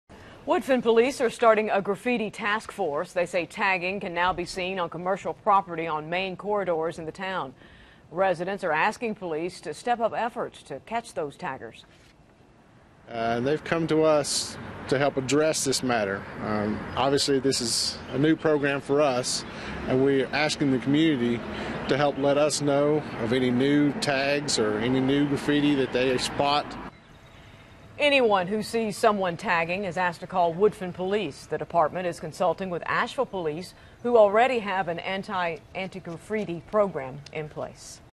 Woodfin PD Forming Graffiti Task Force
BUNCOMBE COUNTY, N.C. -- Woodfin's Police Department has announced the formation of a graffiti task force to address growing concerns from community members about graffiti problems.

"They've come to us to help address this matter," Officer Chris Ammons said, who's heading up the anti-graffiti initiative. "We ask the community to help let us know of any new tags or graffiti that they spot."

Graffiti can be a costly crime for property owners who have to spend sometimes hundreds or thousands of dollars to get tags painted over or signs with graffiti replaced.

Officer Ammons said the department will be consulting with Asheville Police who have a graffiti investigation initiative in place.